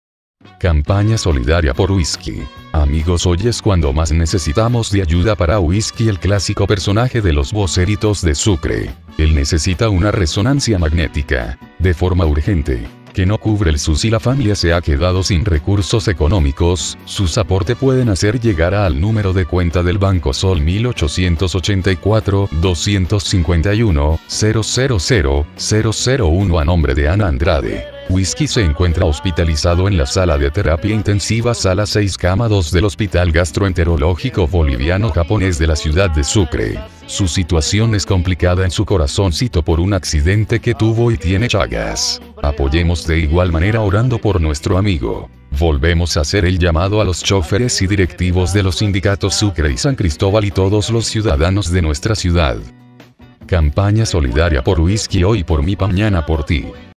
CAMAPAÑA SOLIDARIA POR WISKY APOYO SOCIAL VICTORIA ON LINE TV
CAMPAÑA SOLIDARIA POR WISKY; AMIGOS HOY ES CUANDO MAS NECESITAMOS DE AYUDA PARA WISKY EL CLÁSICO PERSONAJE DE LOS VOCERITOS DE SUCRE; EL NESECITA UNA RESONANCIA MAGNÉTICA; DE FORMA URGENTE QUE NO CUBRE EL SUS Y LA FAMLIA SE HA QUEDADO SIN RECURSOS ECONÓMICOS, SUS APORTE PUEDEN HACER LLEGARA AL NÚMERO DE  CUENTA DEL BANCO SOL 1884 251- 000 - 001 A NOMBRE DE ANA ANDRADE CEL: 77122563; WISKY SE ENCUENTRA HOSPITALIZADO EN LA SALA DE TERAPIA INTENSIVA SALA 6 CAMA 2 DEL HOSPITAL GASTRO ENTEROLÓGICO BOLIVIANO JAPONÉS DE LA CIUDAD DE SUCRE; SU SITUACIÓN ES COMPLICADA EN SU CORAZÓNCITO POR UN ACCIDENTE QUE TUVO Y TIENE CHAGAS; APOYEMOS DE IGUAL MANERA ORANDO POR NUESTRO AMIGO; VOLVEMOS A HACER EL LLAMADO A LOS CHOFERES Y DIRECTIVOS DE LOS SINDICATOS SUCRE Y SAN CRISTOBAL Y TODOS LOS CIUDADANOS DE NUESTRA CIUDAD. CAMPAÑA SOLIDARIA POR WISKY "HOY POR MI PAMÑANA POR TÍ".